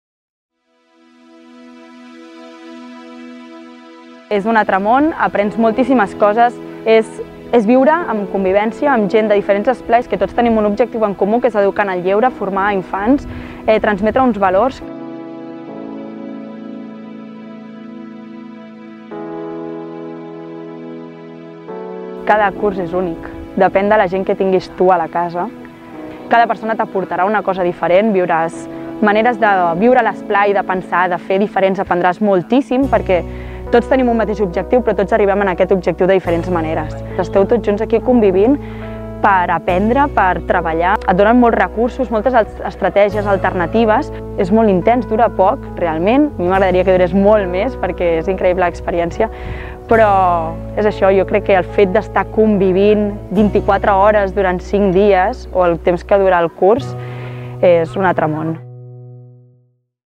MONIS 360º / Curs residencial Setmana Santa / FORMACIÓ MCECC - Fundació Pere Tarrés
Arriba la formació residencial Monis 360º, un curs per a joves que ja tinguin el carnet de monitors/es o directors/es i vulguin aprofundir el coneixement en diverses temàtiques. Un cop t'hagis apuntat hauràs de decidir entre dues opcions, "Eines transversals de l'educació en el lleure" o bé "Coordinació de grups". Del 13 al 16 d'abril a la casa de colònies La Ruca.

Pots inscriure't-hi fins el dia 28 de febrer a través d'aquest enllaç!

Apunta't-hi! T'esperem!